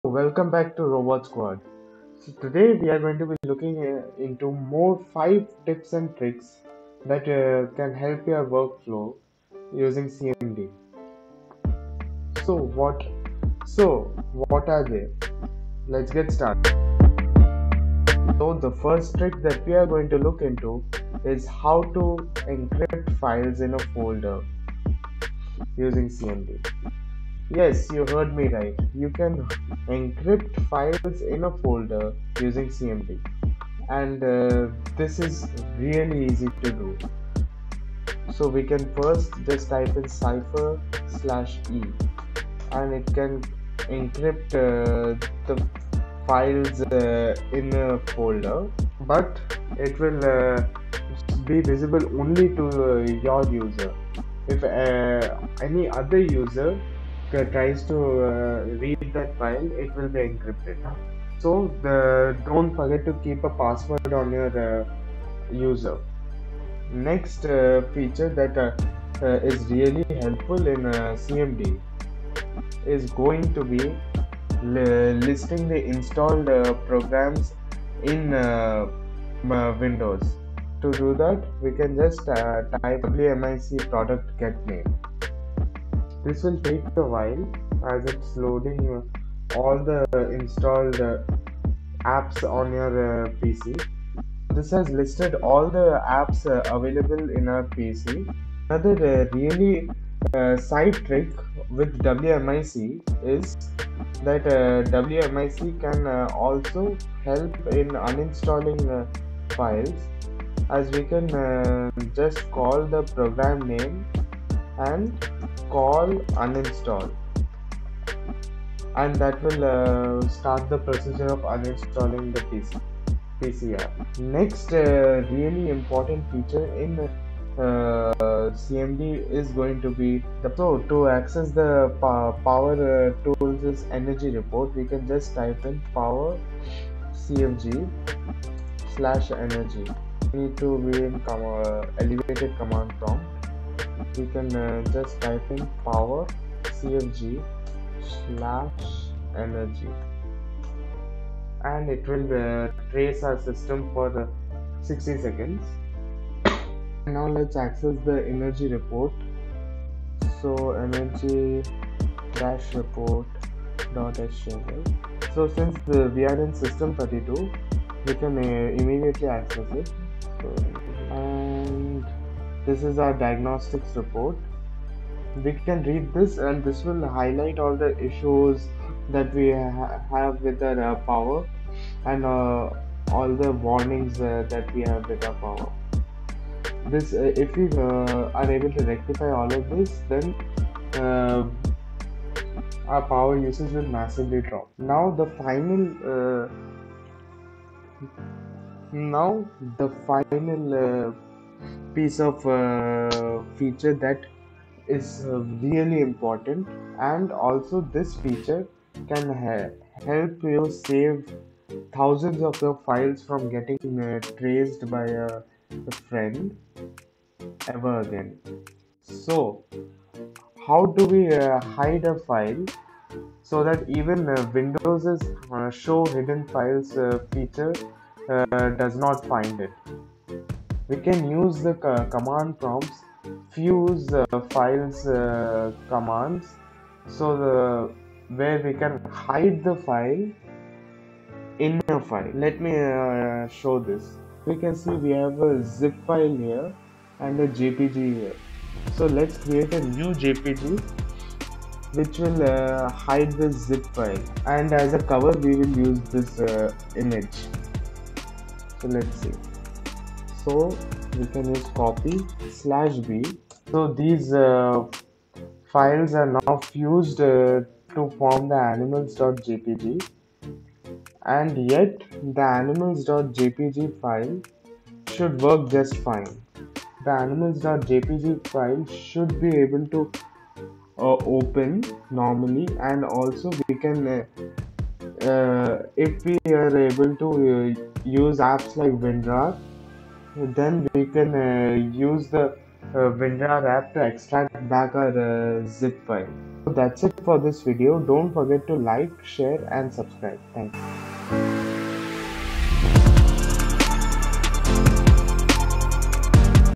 USEFUL CMD HACKS that will BLOW YOUR MIND!!
In this video we cover all the major CMD hacks that will make your workflow more efficient! we also see the various features cmd has to offer us! so join us in this exploration as we find our new hacks and tricks in cmd

*CHAPTERS*
00:22 Encrypt files from command prompt!!
01:11 Listing the installed programs in CMD
01:42 Uninstalling apps using CMD
02:08 The Windows Energy report
03:24 Final feature! (hiding files inside files)